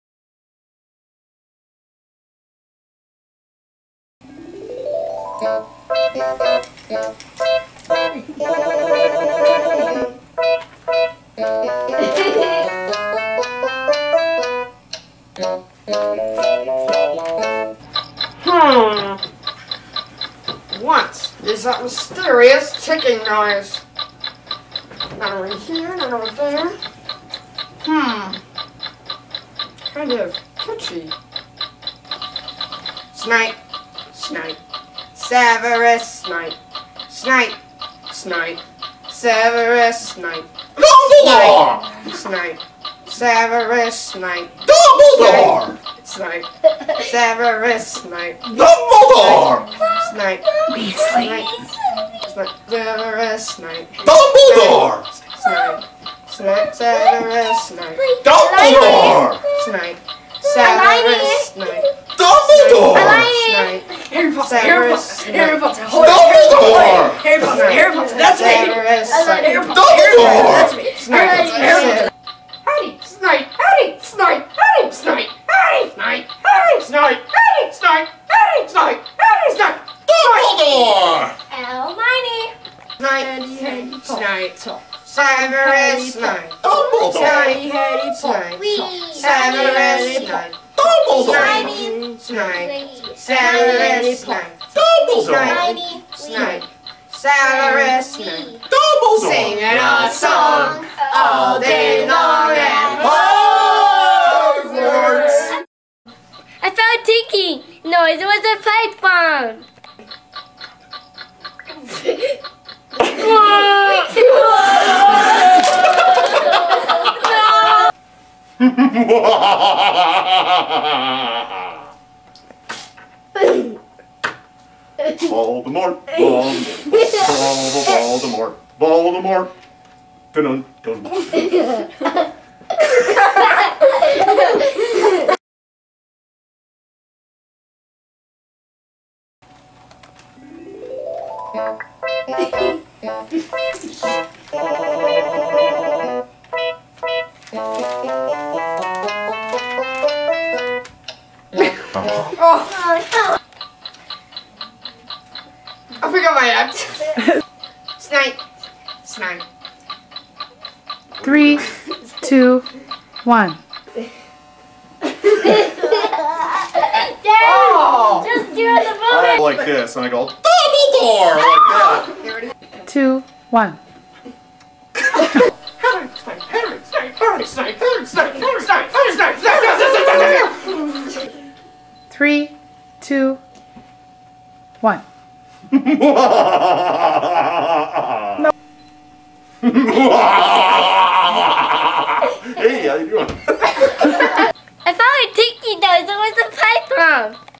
Spoof of Harry Potter and the Mysterious Ticking Noise
A spoof of the most popular Harry Potter video on youtube, called, "What is That Mysterious Ticking Noise?" Recorded at Timberline Lodge in Oregon.  WARNING:  PG-13